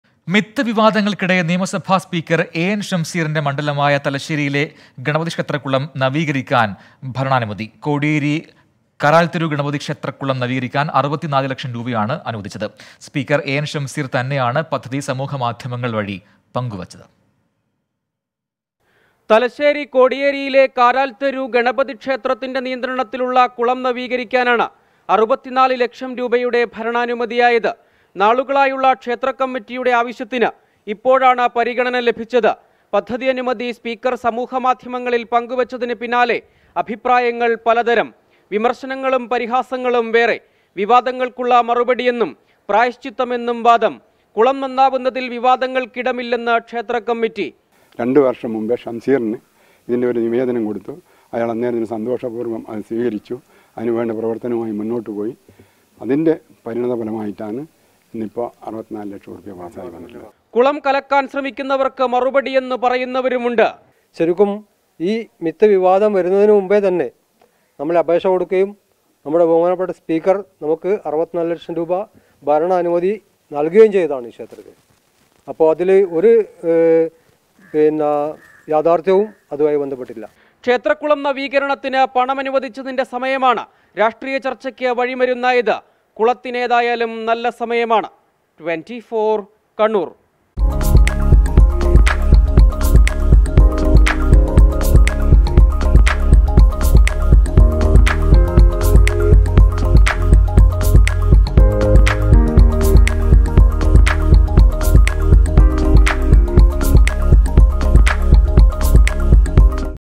എ.എന്‍.ഷംസീറിന്റെ മണ്ഡലമായ തലശ്ശേരിയില്‍ ഗണപതി ക്ഷേത്രത്തിന്റെ കുളം നവീകരിക്കാന്‍ 64 ലക്ഷം
64 lakhs sanctioned for renovation of pond in ganesha temple